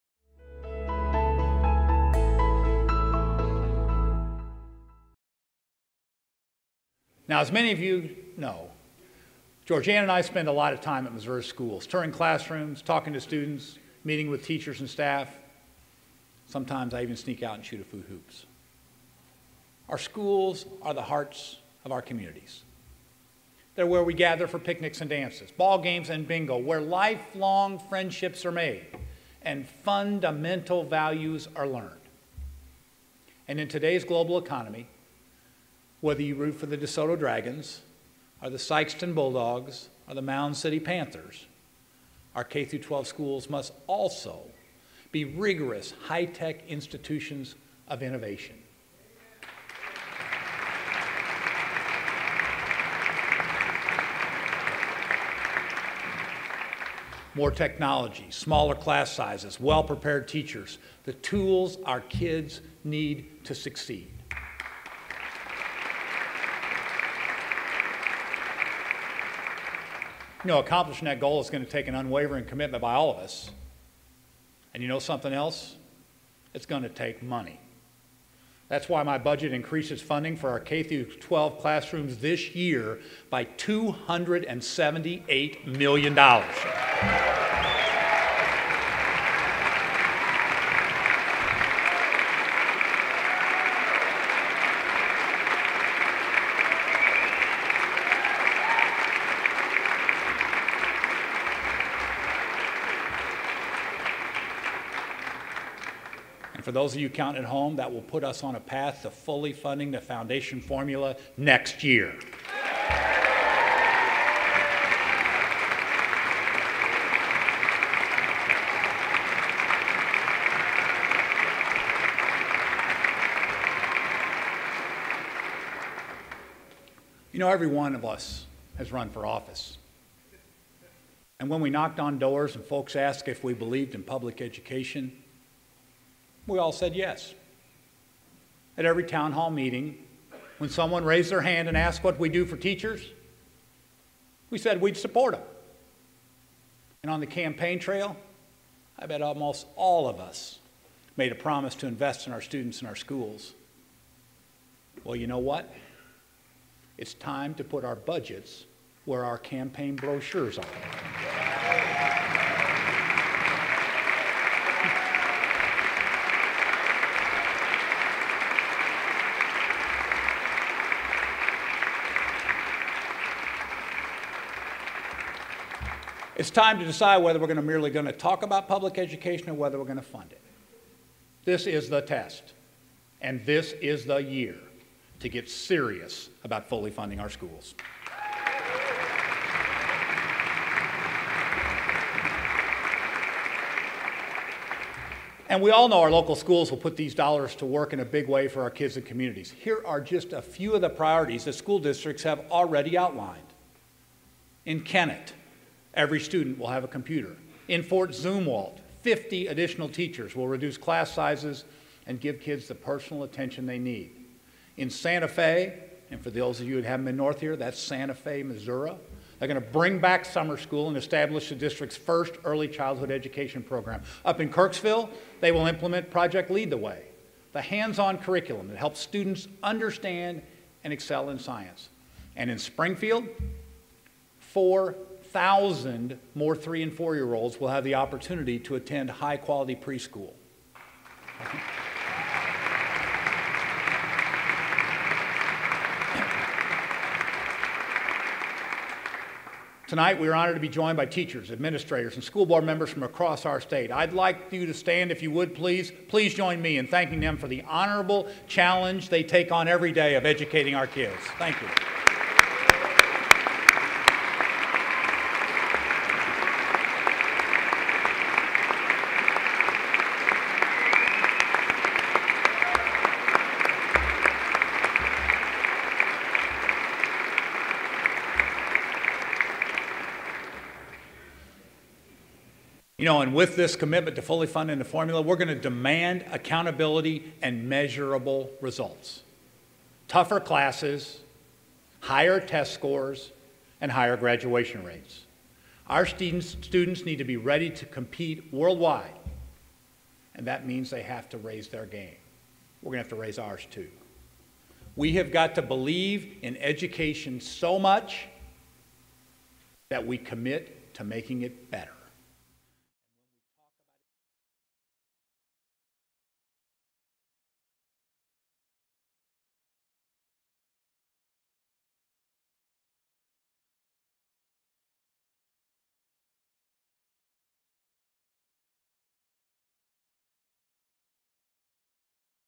State of the State 2014: Funding Education
In his 2014 State of the State Address, Gov. Jay Nixon detailed his plan to build on the momentum in Missouri's economy by investing in students and schools.  From preschool to graduate school, Gov. Nixon's budget includes $493 million for education.

Read Gov. Nixon's complete State of the State Address